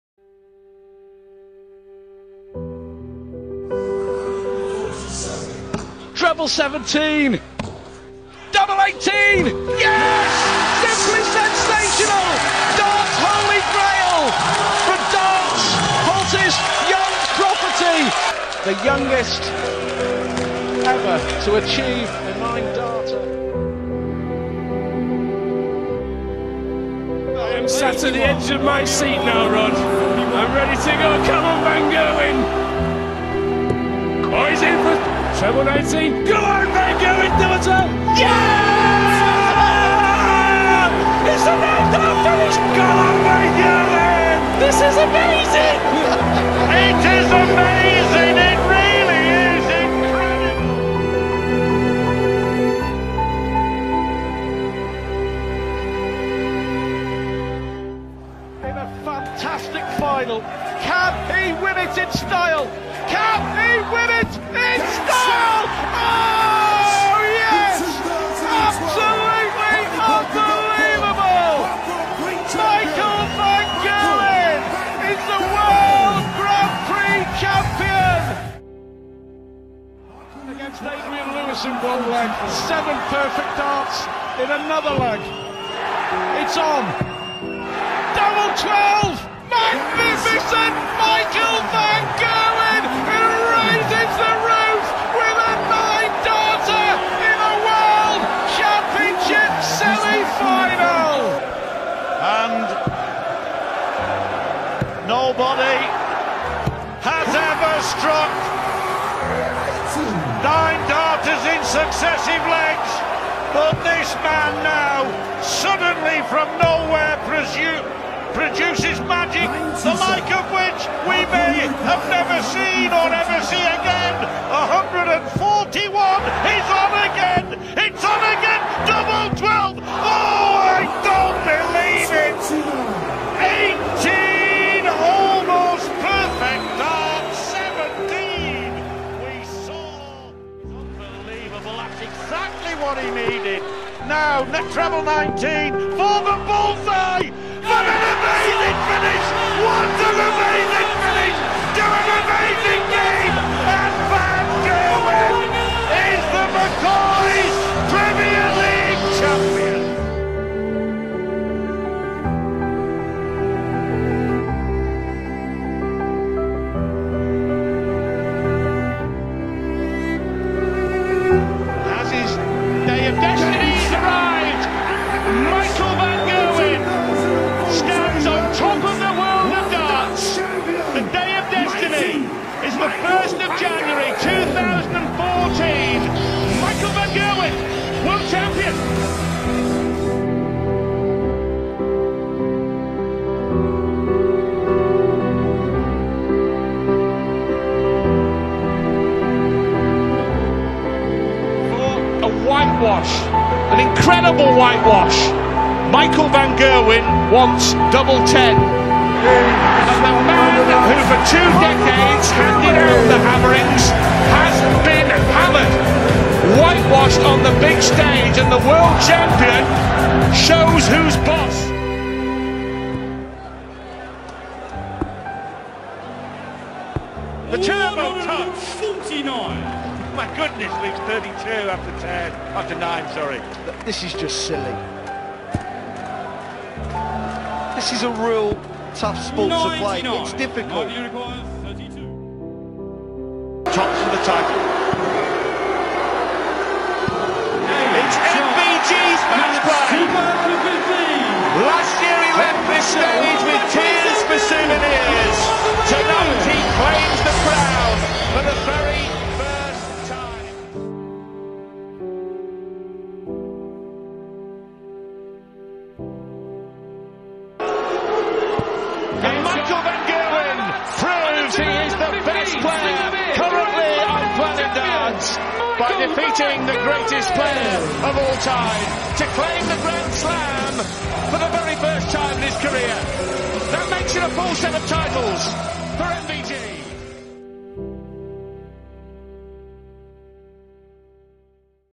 Michael Van Gerwen | Darting Phenomenon
MVG is simply unstoppable, in terms of, he will not let anything get in his way of his dream to become the greatest darts player of all time. At 26 the dutchman has already won the lot, every PDC major, the youngest, and only other, from Phil Taylor to achieve this wonderful feet.